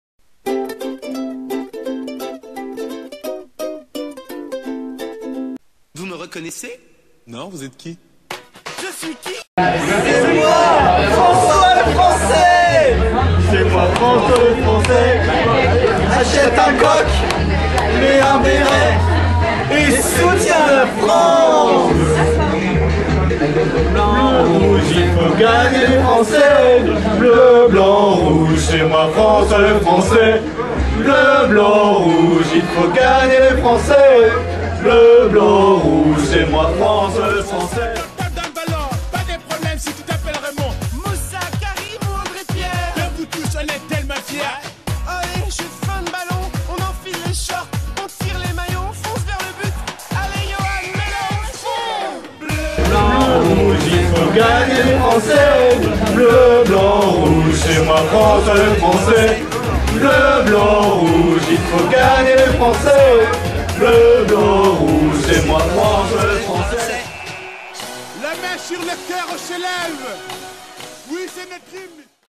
PLIZ 2010 Défi 1 Bleu, blanc, rouge dans un pub irlandais
Défis insolites réalisés par les membres de l'association La Muse Gueule, au cours du PLIZ 2010. Les dons générés par la réussite de ces défis ont été reversés à "Toupar' du Coeur" qui oeuvre pour les sans abris à Paris et agit en Haïti.